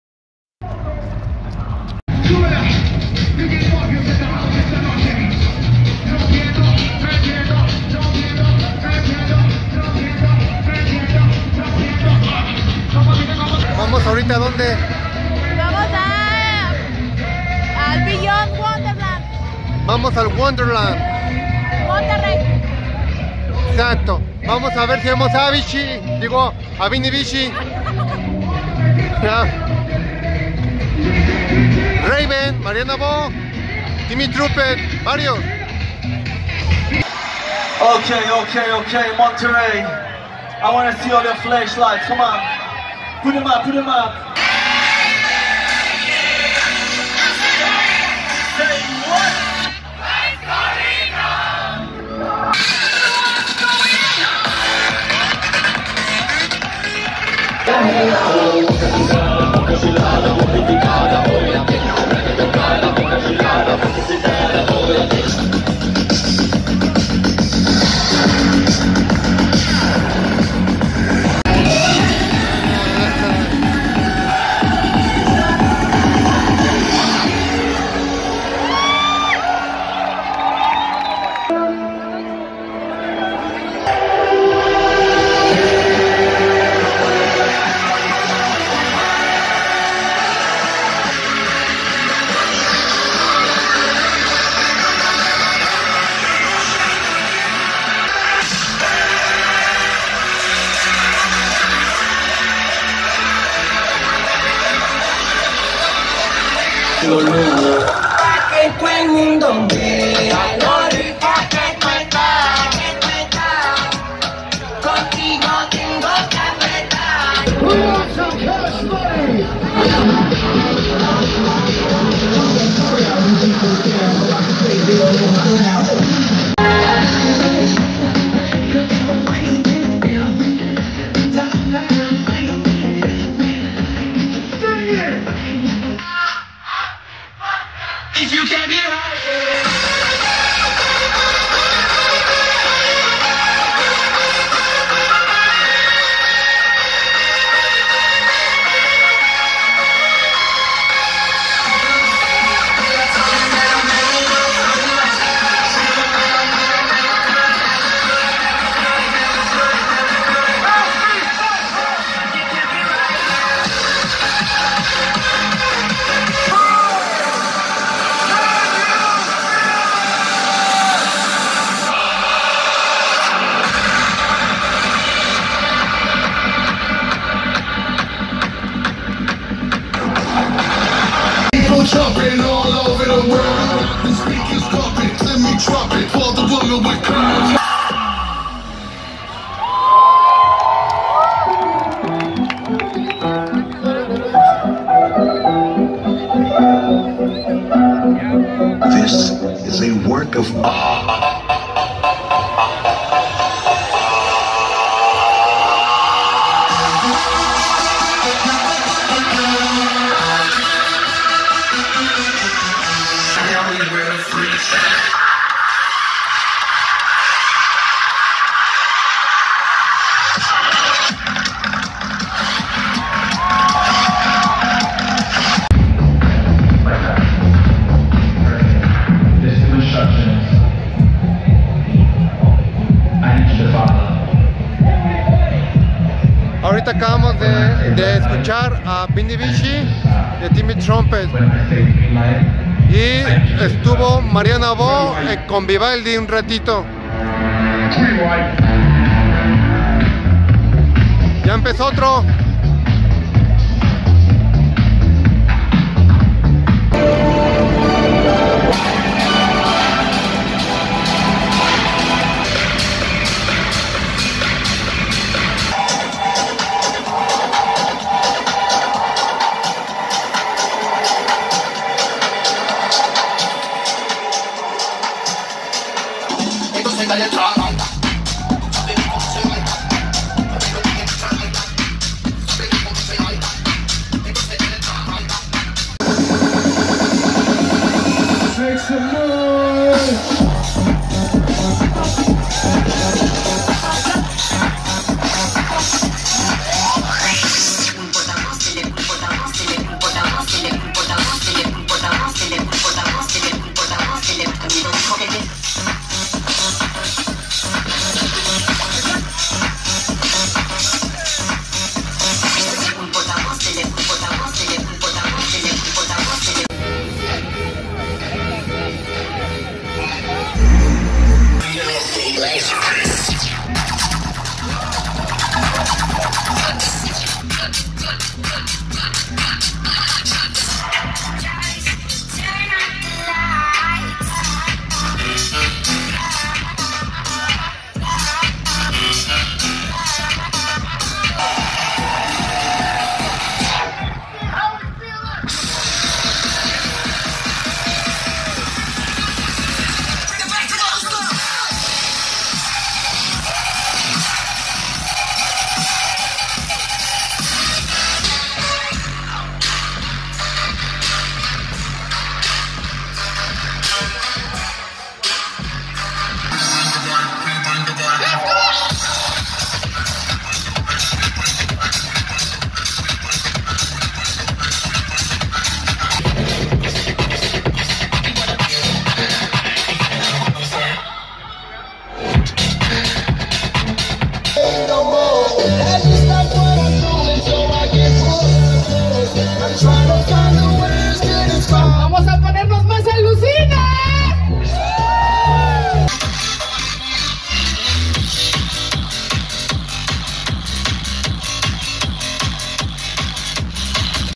Beyond Wonderland 2022 MTY | Vlog Resume
Espero que este video les haya gustado como a mi haberlo hecho.

NOS VEMOS MUY PRONTO CON MAS VIDEOS .

Este Beyond Wonderland fue en parque fundidora Monterrey.